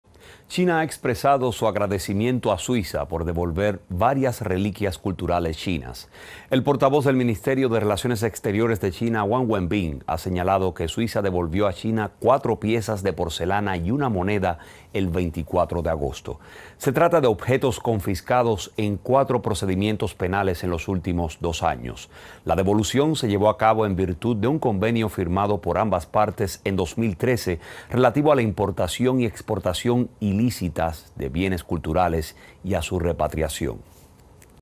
China expresa su agradecimiento a Suiza por la devolución de varias reliquias confiscadas
China ha expresado su agradecimiento a Suiza por devolver varias reliquias culturales chinas. El portavoz del Ministerio de Relaciones Exteriores de China Wang Wenbin ha señalado que Suiza devolvió a China cuatro piezas de porcelana y una moneda el 24 de agosto. Son objetos confiscados en cuatro procedimientos penales en los últimos dos años. La devolución se llevó a cabo en virtud de un convenio firmado por ambas partes en 2013, relativo a la importación y exportación ilícita de bienes culturales y a su repatriación.